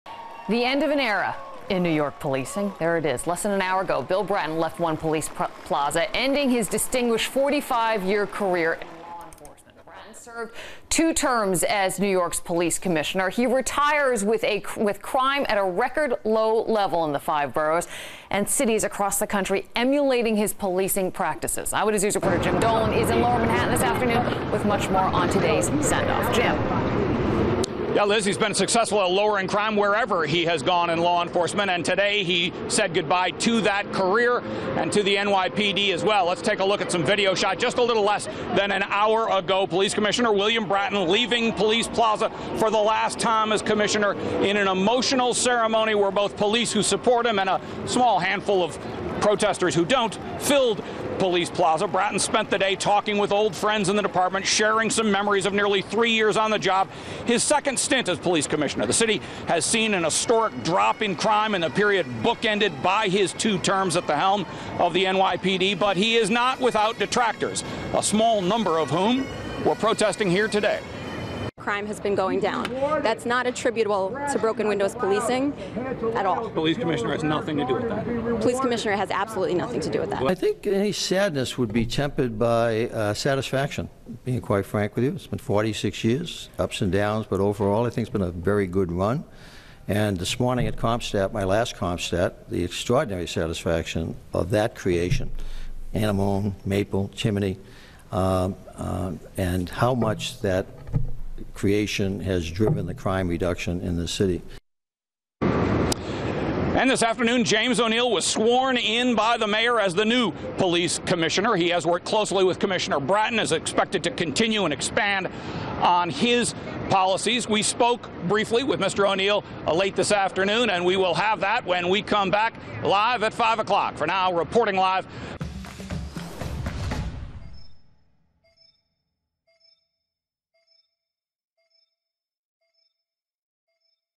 Bill Bratton Retires as NYPD Commissioner
New York City Police Commissioner Bill Bratton retired today, leaving One Police Plaza in a ceremonial walk-out that marked the end of his second time serving as the leader of the nation's largest police force.